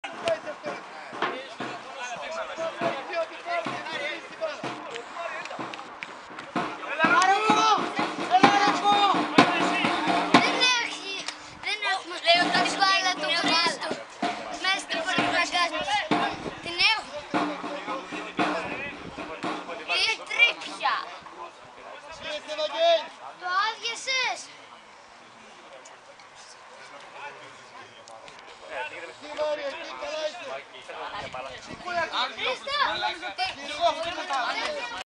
Μαλιακός-Πελασγιακός 6-2, Φάση 39η, Φιλικός Αγώνας, Σαβ 16 Σεπ 2017 DSCN6446
Φιλικός αγώνας μεταξύ του Μαλιακού και της ομάδας μας, που διεξήχθη στο στάδιο Στυλίδας, το απόγευμα του Σαββάτου 16 Σεπ 2017, με σκόρ 0-0 στο ημίχρονο και 6-2 τελικό. Τα γκόλ για την ομάδα μας οι Μάνος Χάρης και Ράπτης Αντώνης Κ.